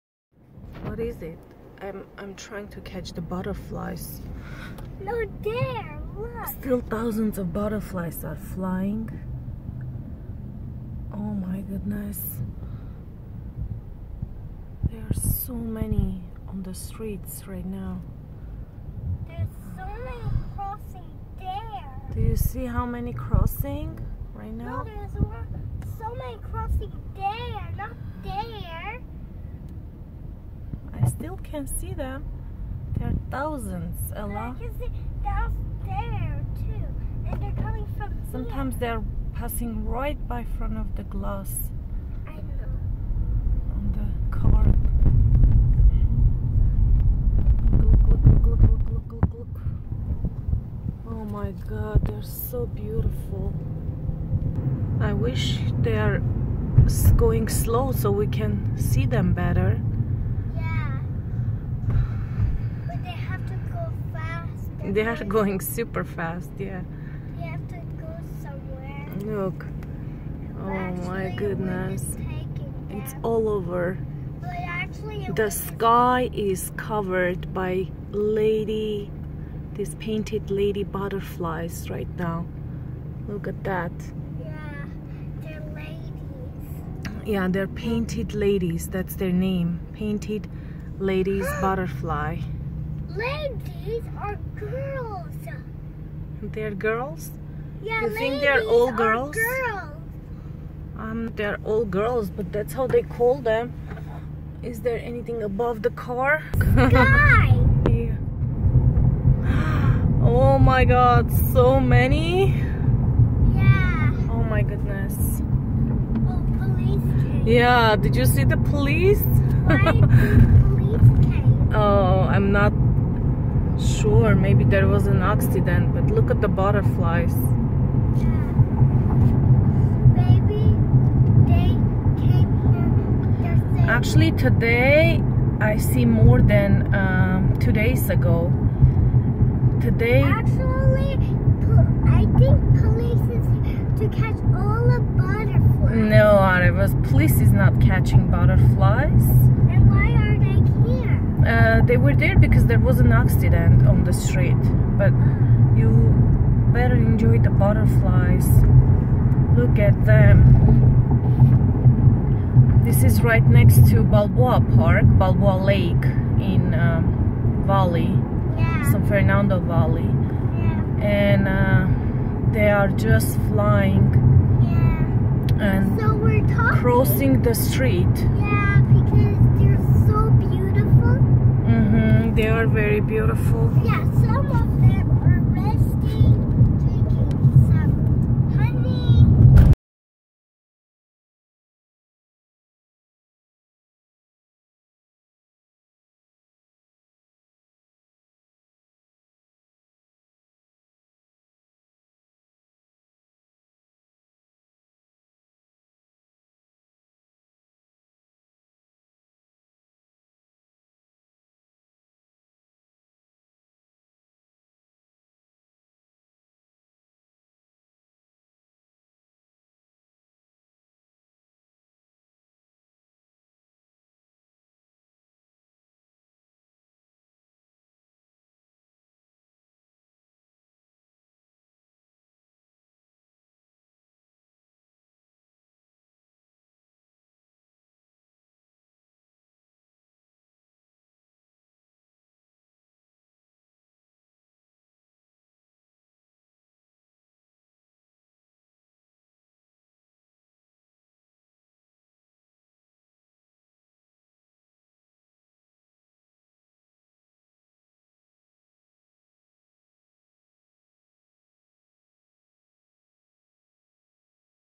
Painted Lady Butterflies all over San Fernando Valley
I was driving to Encino through Burbank Blvd. near Balboa lake park,  and the streets were full of beautiful painted lady butterflies. March 16, 2019